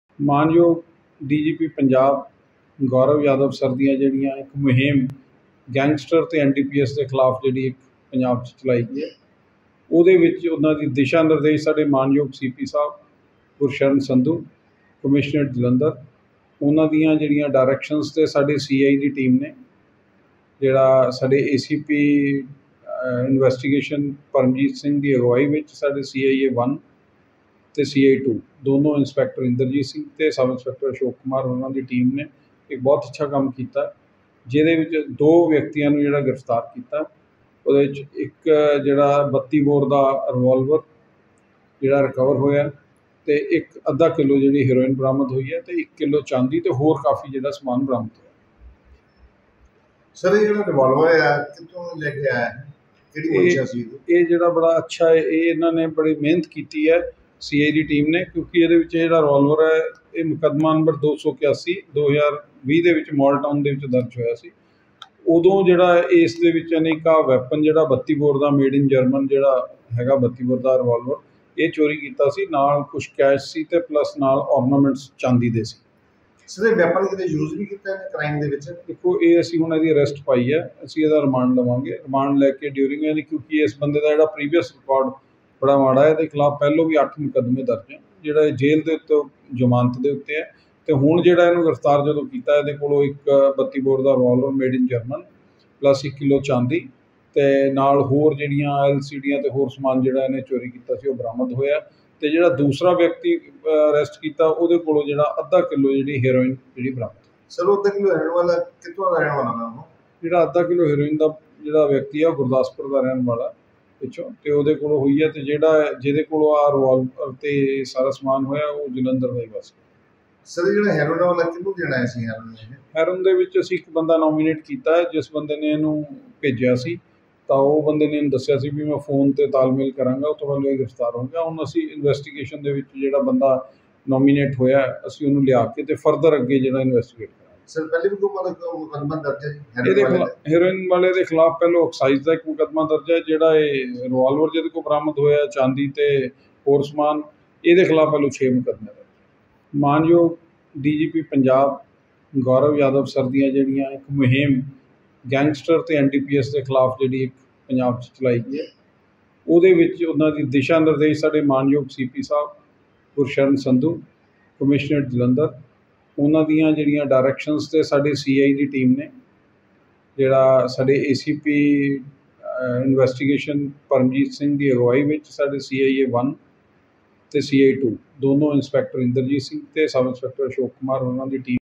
A big success in the hands of the police, 2 accused who committed robberies were arrested
A big success in the hands of the police, 2 accused who committed robberies were arrested, 32 bore German revolver was recovered

ਜਲੰਧਰ : ਪੁਲਿਸ ਹੱਥ ਲੱਗੀ ਵੱਡੀ ਸਫਲਤਾ, ਲੁੱਟਾਂ-ਖੋਹਾਂ ਕਰਨ ਵਾਲੇ 2 ਮੁਲਜ਼ਮ ਕੀਤੇ ਕਾਬੂ, 32 ਬੋਰ ਦਾ ਜਰਮਨ ਰਿਵਾਲਵਰ ਕੀਤਾ ਬਰਾਮਦ

Punjabi Bulletin